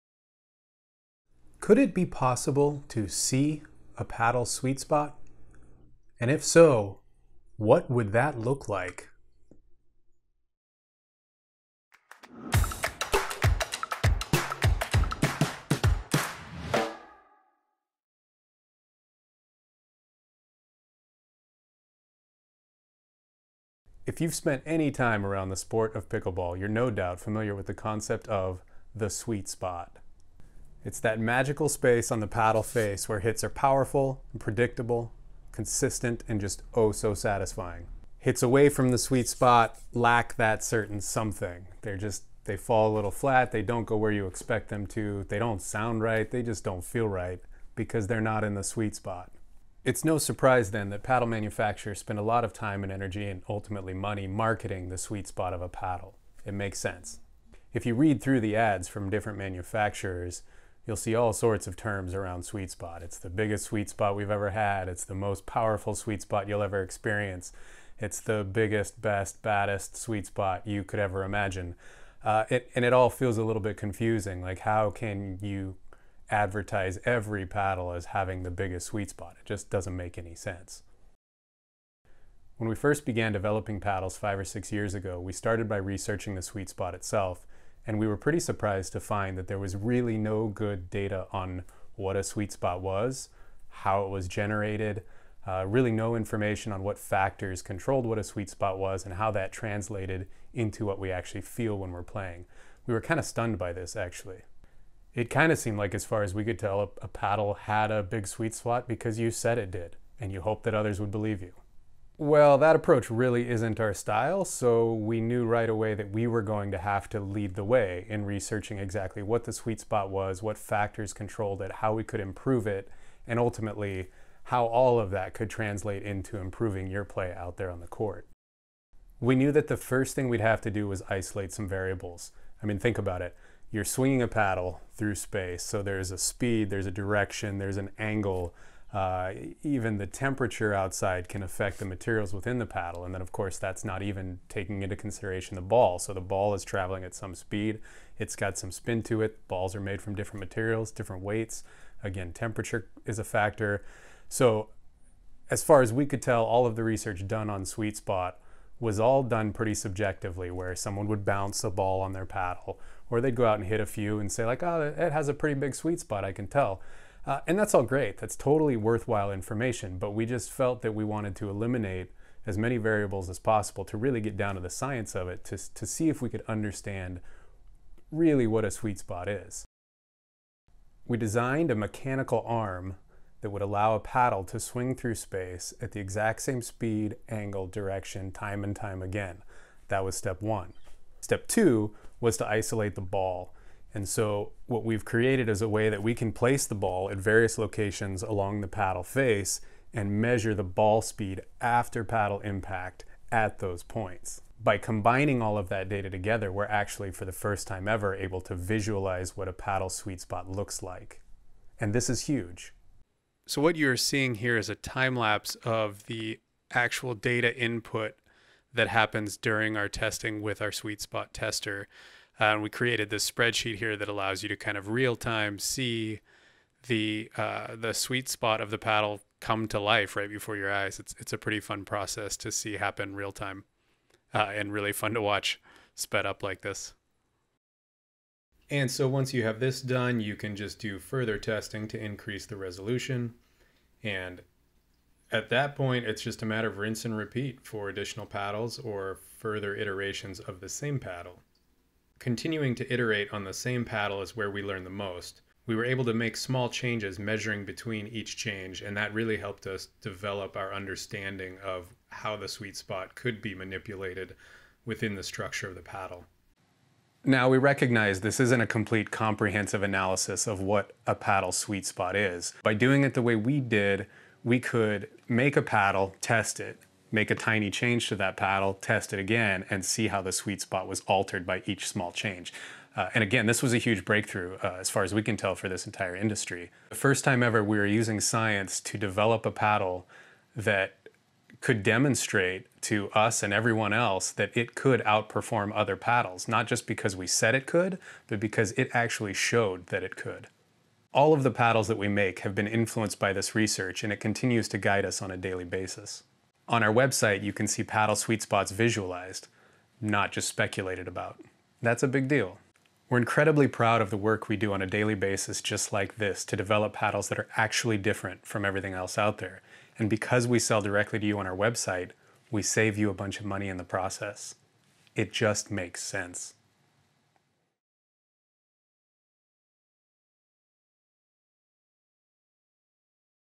Pickleball Paddle Sweet Spot Science
Could it be possible to "see" the sweet spot on a paddle? If so, what would it look like?

See the efforts Players Pickleball made to provide a new insight into that wonderful area of every paddle known as the "sweet spot"!